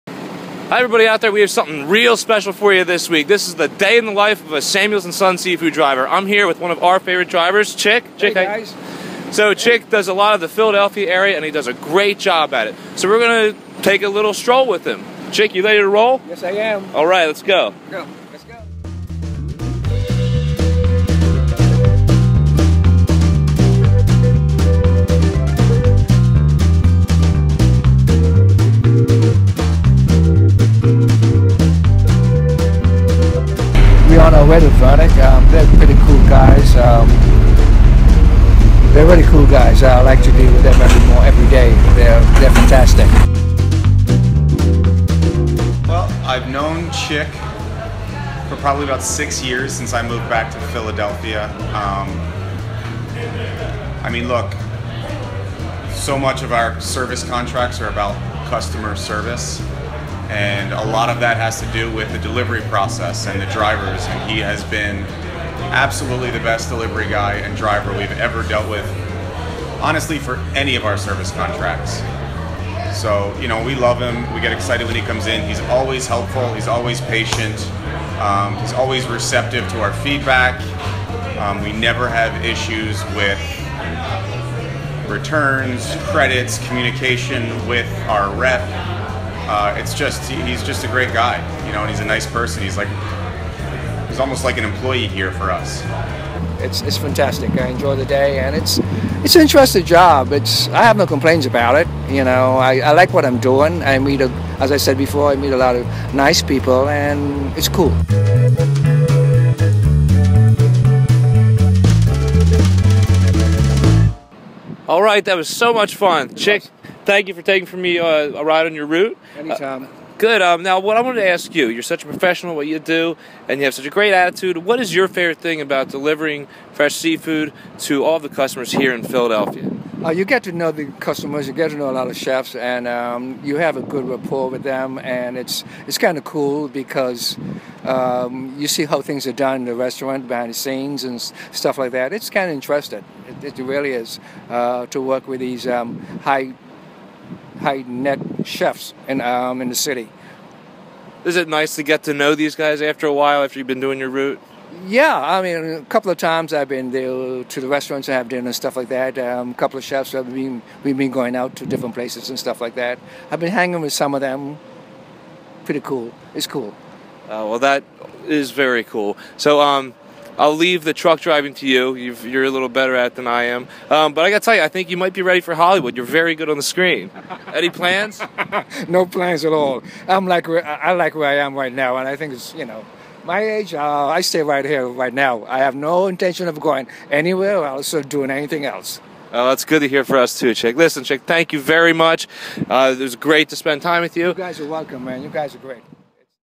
A Day In The Life: Chick Looby, Driver
We have a special treat for our viewers this week! We were able to tag along with Chick Looby, a driver here at Samuels Seafood, while he made a delivery along his route. Come watch to see a sneak peek into the life of a driver at Samuels Seafood.